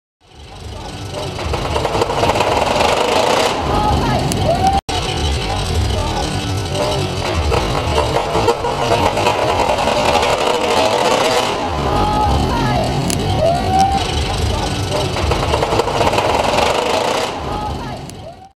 Colossus Lift Hill Sound
The famous Lift Hill Sound heard on Colossus at Thorpe Park as the Train begins to detach from the Chain! Enjoy!